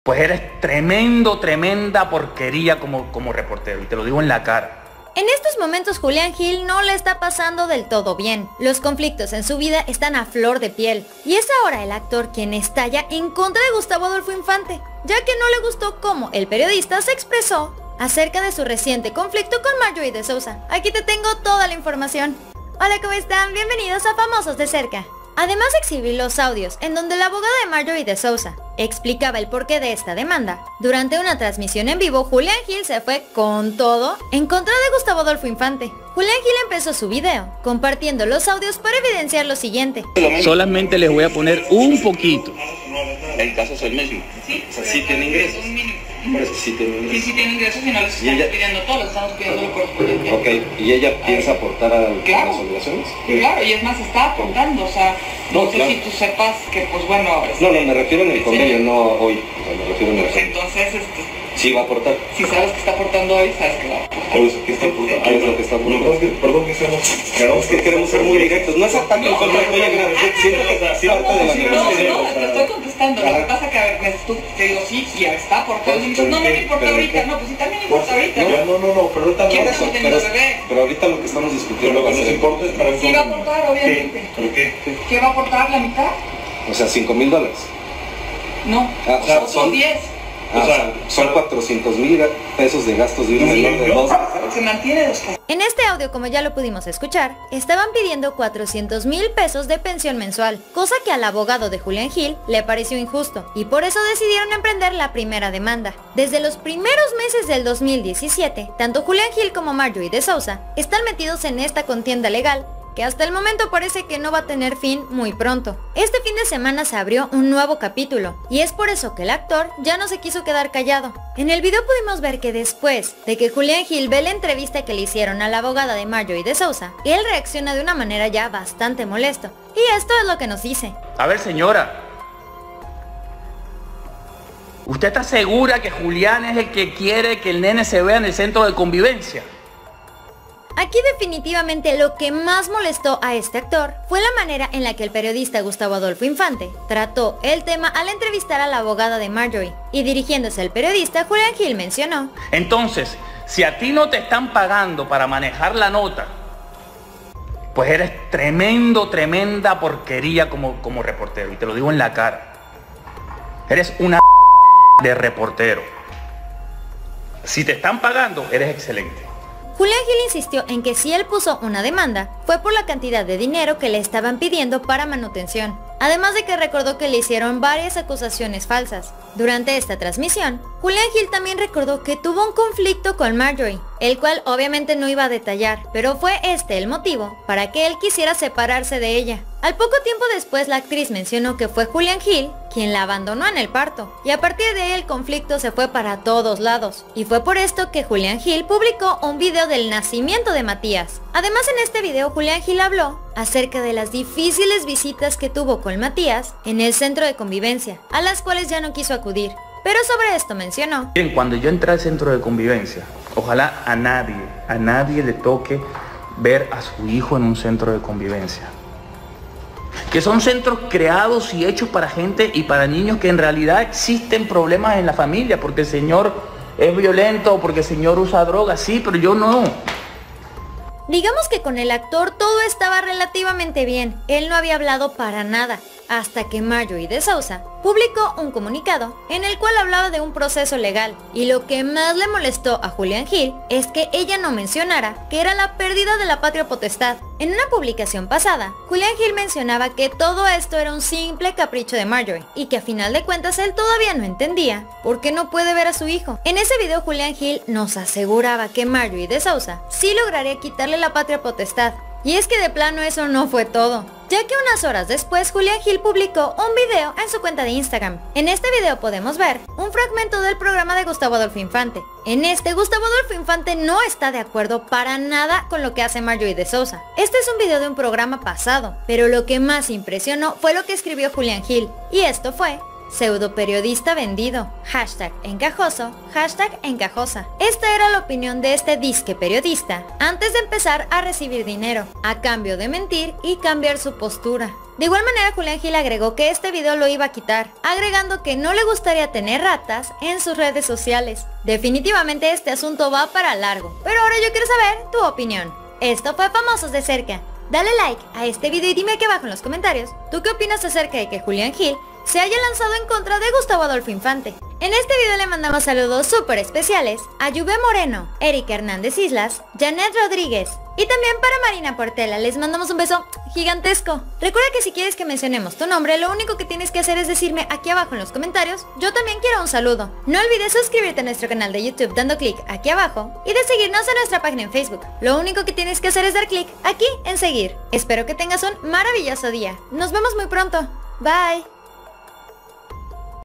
⛔😤 ¡ 𝗝𝘂𝗹𝗶𝗮𝗻 𝗚𝗶𝗹 𝗣𝗼𝗻𝗲 𝗲𝗻 𝘀𝘂 𝗟𝘂𝗴𝗮𝗿 a Gustavo Adolfo 𝗣𝗼𝗿 𝗦𝘂 𝗙𝗮𝗹𝘁𝗮 𝗱𝗲 𝗣𝗿𝗼𝗳𝗲𝘀𝗶𝗼𝗻𝗮𝗹𝗶𝘀𝗺𝗼 ! 🚨⚠️
𝗘𝗹 𝗙𝗮𝗺𝗼𝘀𝗼 𝗔𝗰𝘁𝗼𝗿 𝗝𝘂𝗹𝗶𝗮𝗻 𝗚𝗶𝗹  𝗹𝗲 𝗱𝗶𝗰𝗲 sus verdades a Gustavo Adolfo Infante.

 BIENVENIDOS!!!